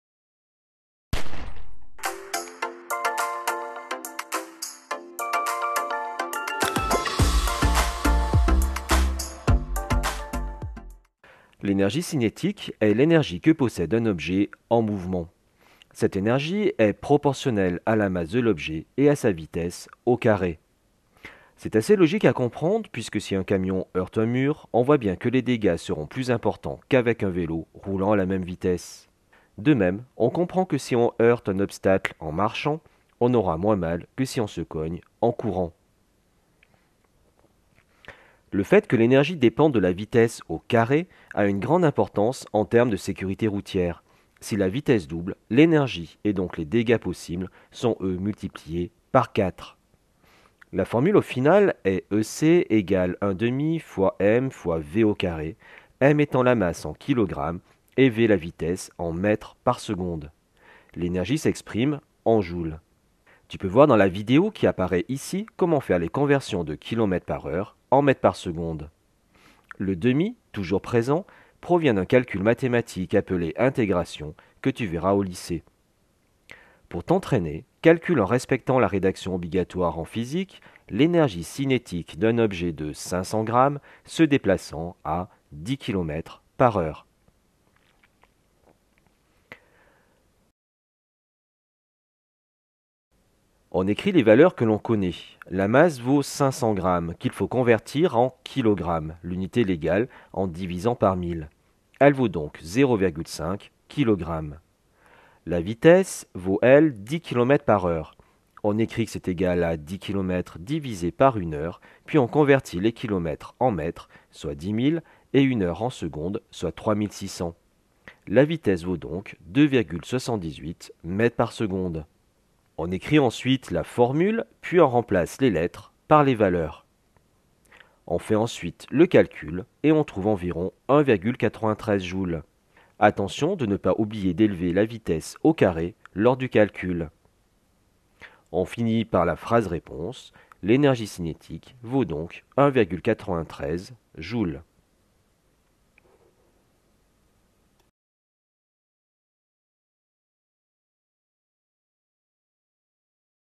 énergie cinétique version rapide 🚀 : mécanique collège et lycée (niveau 3ème et plus)
🚀📐 Qu'est-ce que l'énergie cinétique. Comment calculer l'énergie cinétique d'un objet en mouvement ? Masse, énergie, vitesse. Exercice corrigé.